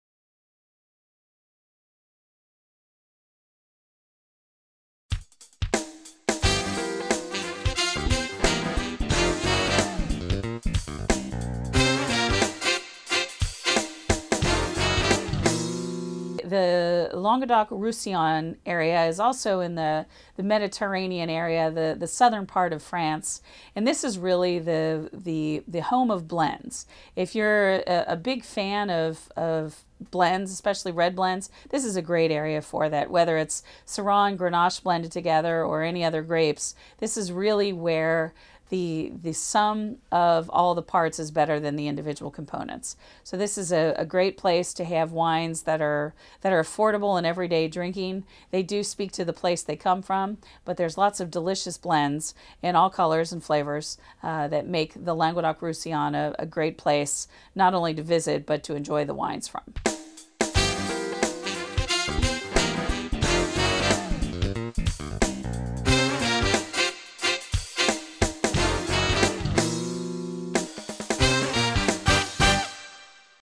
Wine Passport - Languedoc-Roussillon Region | Mondays with Mary Ep. 216
Break out your WinePassport and join Heidelberg Distributing VP of Fine Wine & Education and Advanced Sommelier Mary Horn on a wine tour of the Languedoc-Roussillon Region in France.

France's largest wine region at around 1,200 square miles, Languedoc-Roussillon stretches from the Rhone Valley in the east to the Spanish border in the southwest. It is know for great blends and exceptional value. Join us as we explore the Languedoc-Roussillon Region on this week's Mondays with Mary!

Mondays with Mary
8.20.18